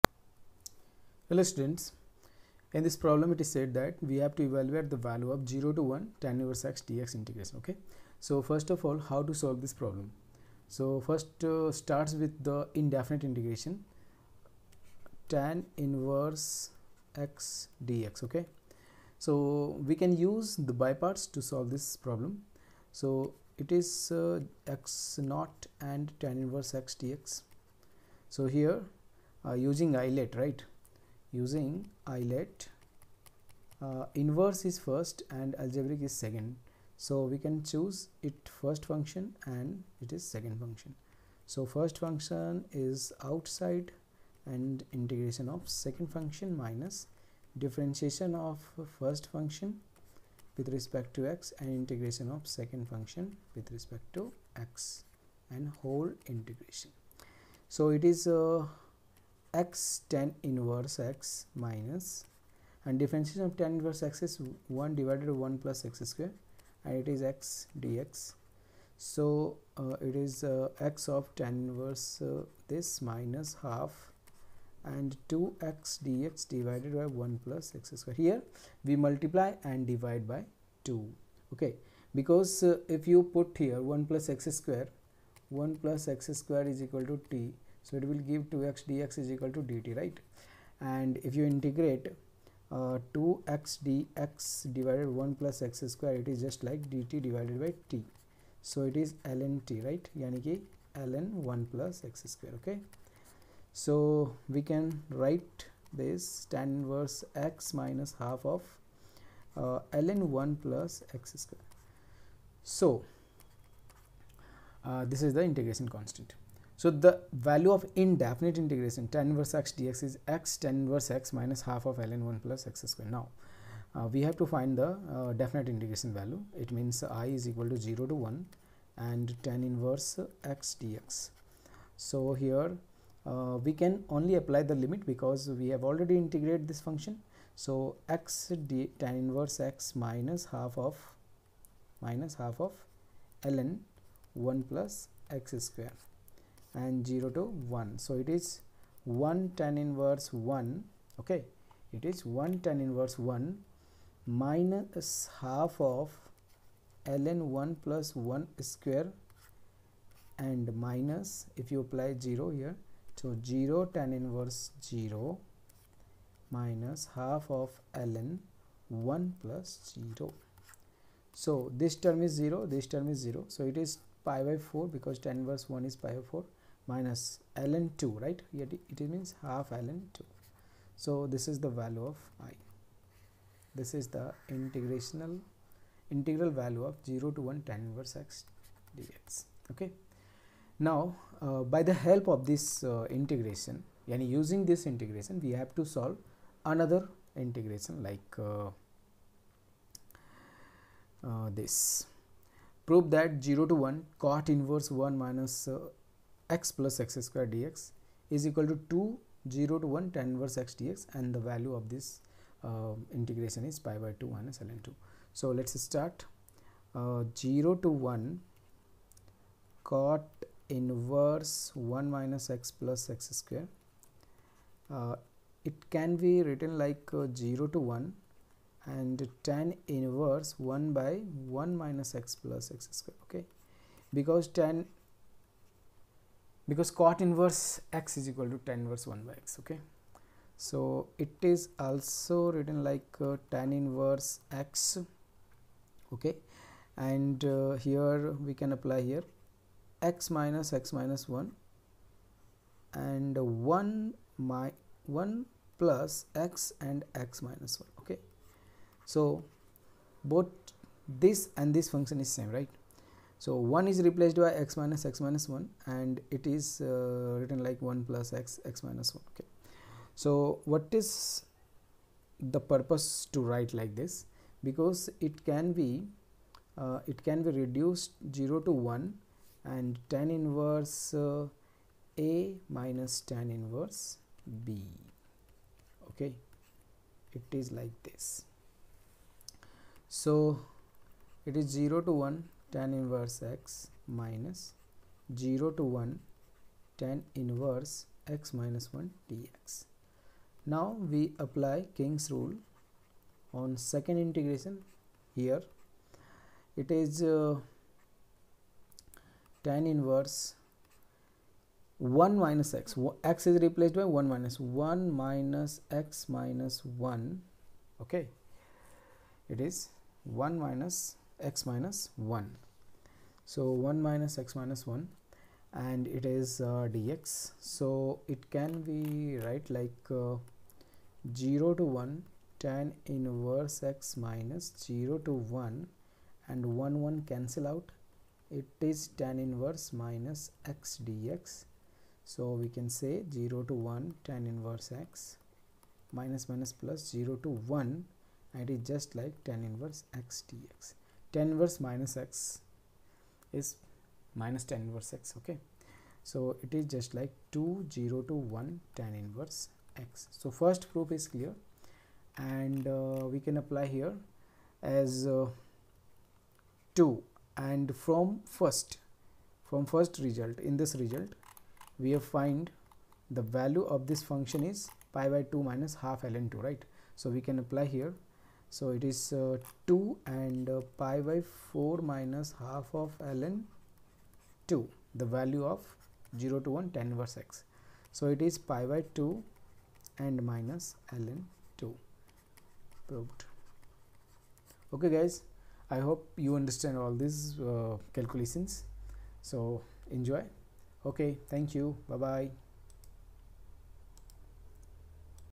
Integrate the function arccot(1-x+x^2) from 0 to 1
Definite Integration for class 12th, JEE, ISI, CMI, CET.